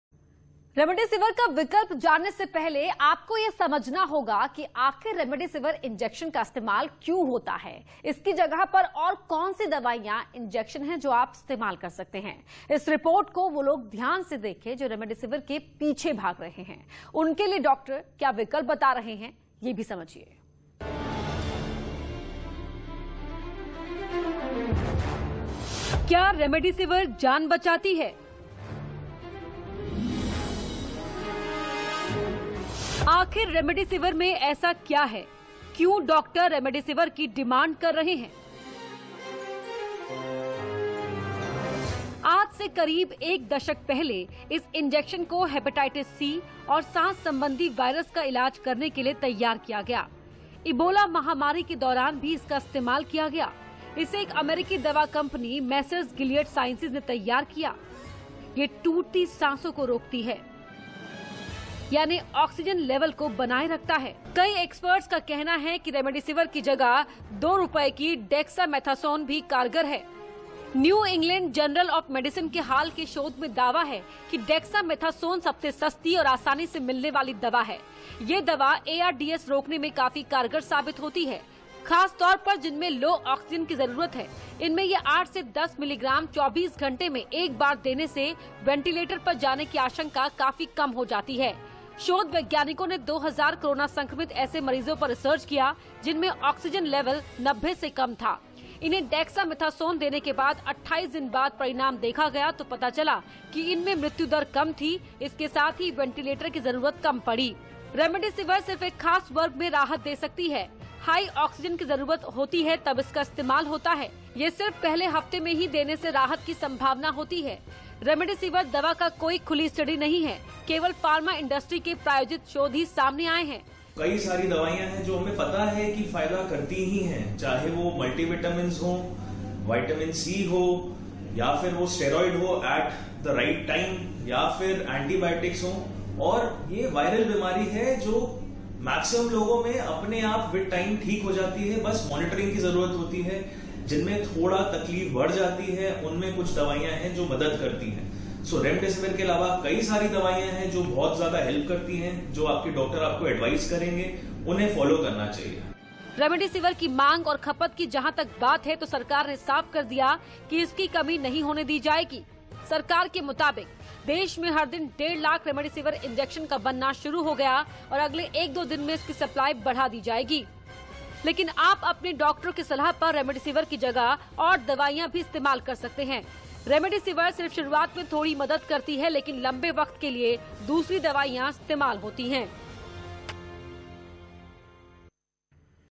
Does Remdesivir work in COVID treatment | Dr. Akshay Budhraja on News18
Dr. Akshay Budhraja, Consultant, Respiratory and Sleep Medicine, Aakash Healthcare Super Speciality Hospital, Dwarka, talked about the usage of medicine Remdesivir in treatment of the Novel Coronavirus.